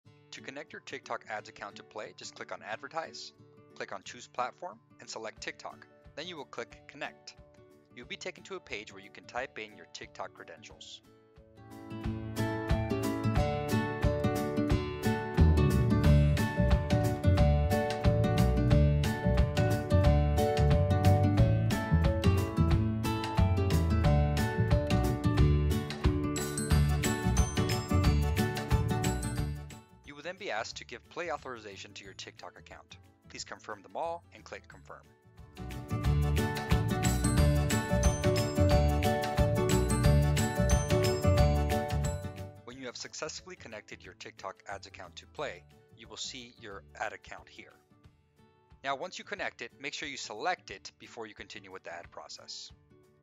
How To Easily Create TikTok Ads By Connecting Your Ad Account To Plai
Before you run TikTok ads on Plai, you'll need to create a TikTok ad account ( see our video here on how to create a TikTok ad account and link your ad account to Plai. Here are the steps on how to link your TikTok Ad account to Plai.

To easily create better TikTok ads, link your ad account to Plai